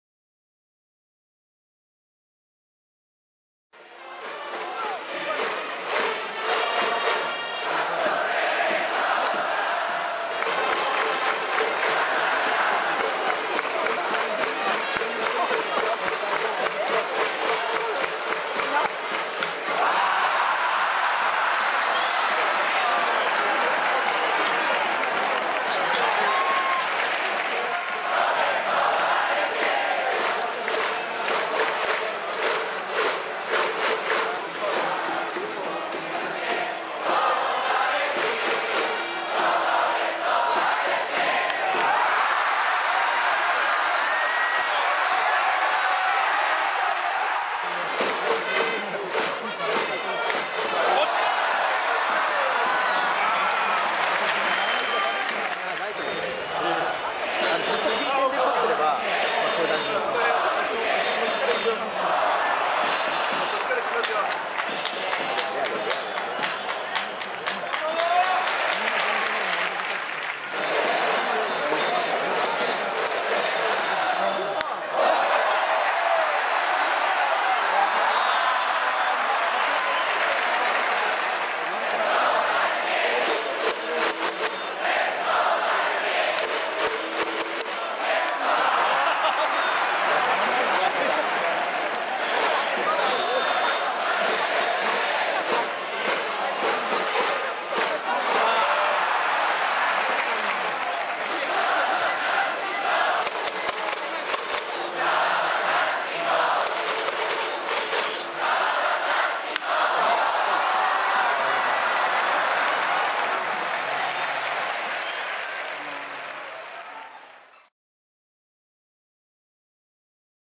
巨人 内海 投球ダイジェスト 2015 CS S-G 2015.10.14
2015セ・リーグクライマックスシリーズ
ファイナルステージ第一戦
ヤクルトvs巨人より。

撮影機材
OLYMPUS OM-D E-M5
Panasonic45-175mm
編集